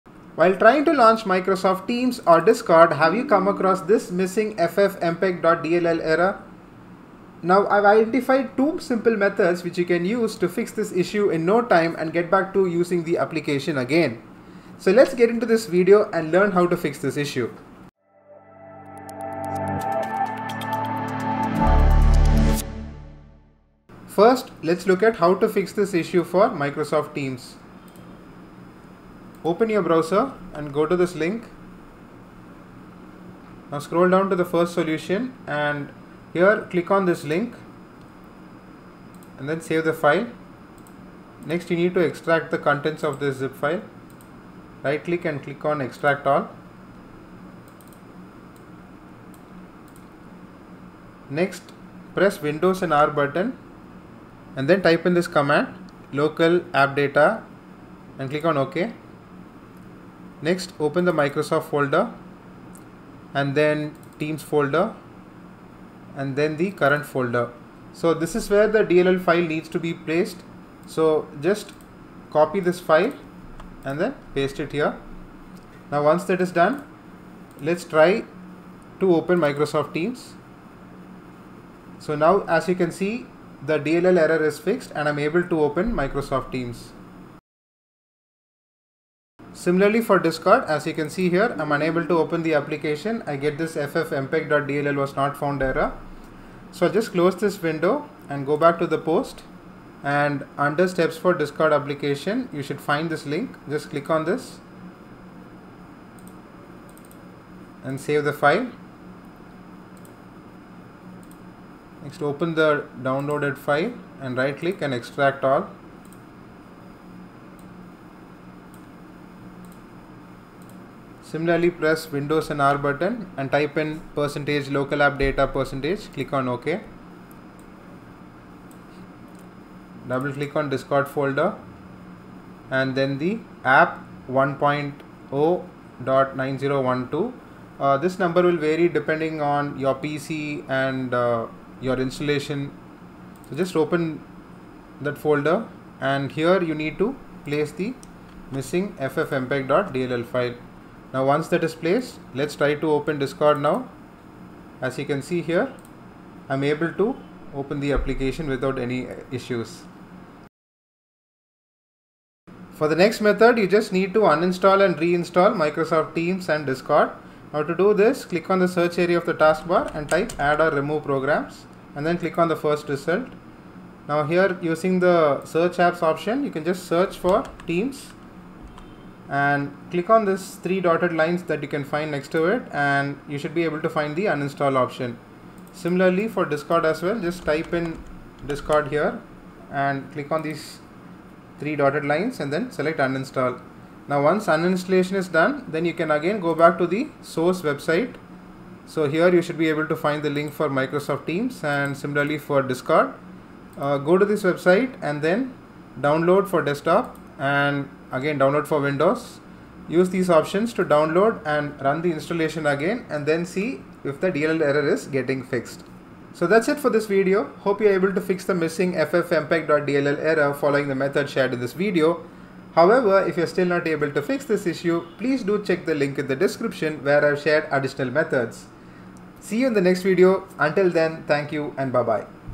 How to Fix ffmpeg.dll Missing Error - Teams & Discord
This is a complete tutorial on how to fix FFMPEG.DLL not found error. These methods are applicable on Windows OS versions – 11, 10, 8.1, 8, and 7.

Contents of Video:
00:00 – Introduction
00:23 – Fix for Microsoft Teams
01:27 – Fix for Discord
02:40 – Method 2
03:36 – Outro